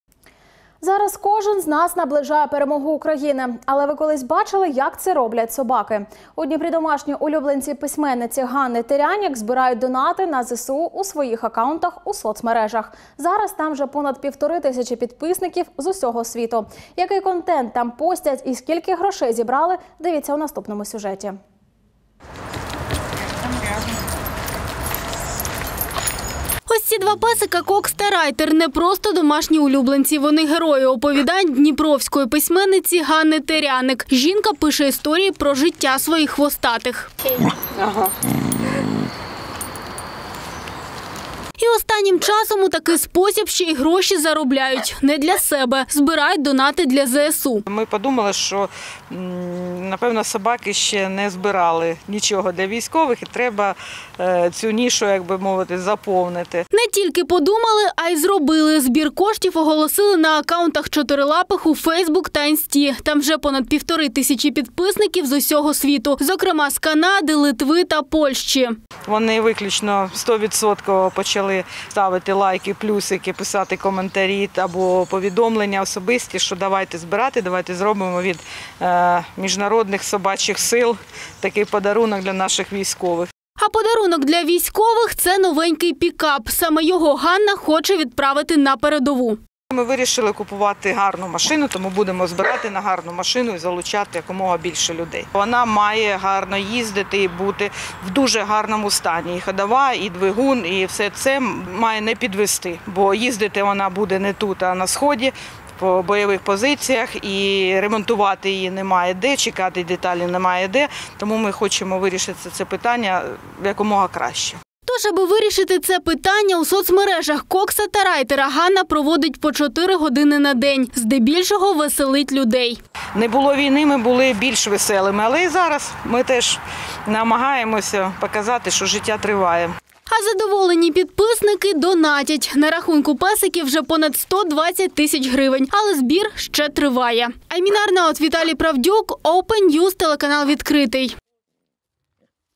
У Дніпрі двоє собак на власних сторінках у соцмережах збирають на автівку військовим
Зараз кожен з нас наближає перемогу України. Але ви колись бачили, як це роблять собаки? Який контент там постять, і скільки грошей зібрали? Дивіться у наступному сюжеты _______________________________________________